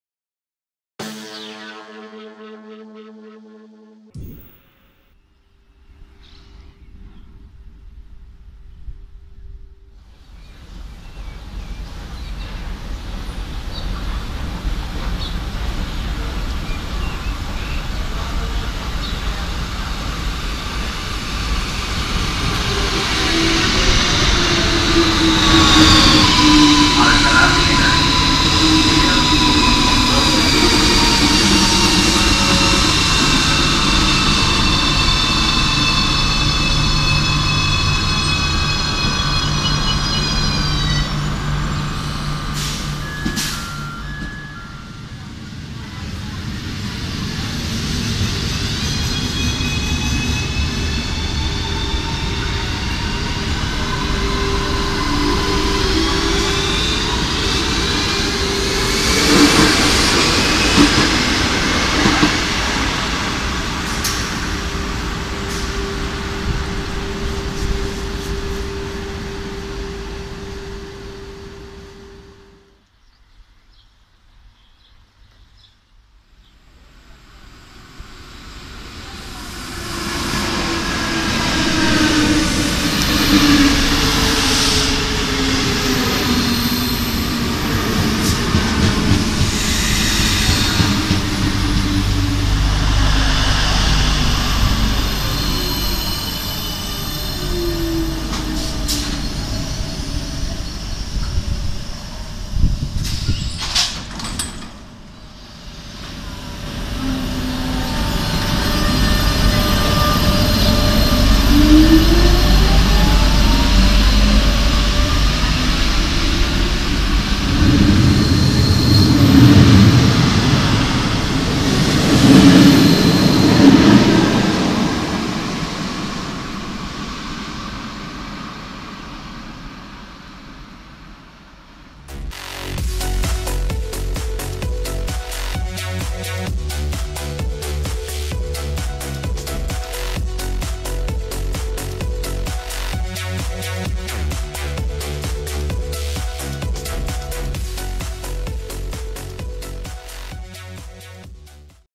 Coslada C2 - C7 : Cercanías Madrid ( Renfe Civia - 446 )
Estación de Coslada de Cercanías Madrid ( C2 - C7 ). Trenes Renfe Civia y 446.

- Estación inaugurada en los años 80.
- Tiene correspondencia con la estación de Coslada Central de la línea 7 del metro de Madrid.
- La estación se encuentra entre las estaciones de Vicálvaro y San Fernando, en el municipio de Coslada. La estación es propiedad de Adif.

Music

ACC84 (2018)